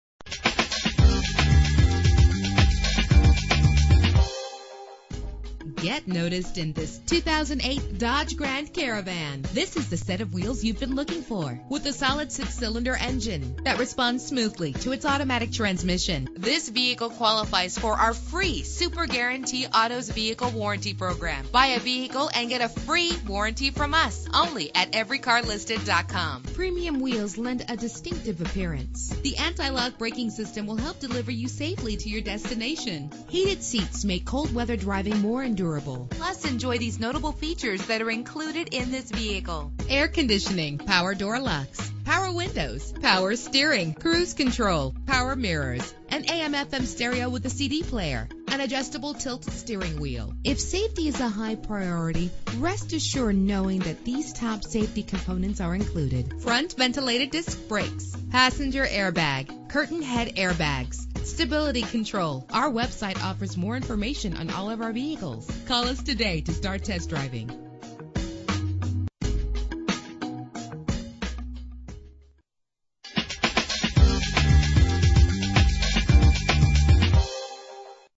2008 Dodge Grand Caravan - Gurnee IL
Year: 2008
Make: Dodge
Model: Grand Caravan
Trim: SXT
Engine: 3.8 liter 6 cylinder 12 valve
Transmission: Automatic
Color: Black
Mileage: 23637
Address:
18834 W Grand Ave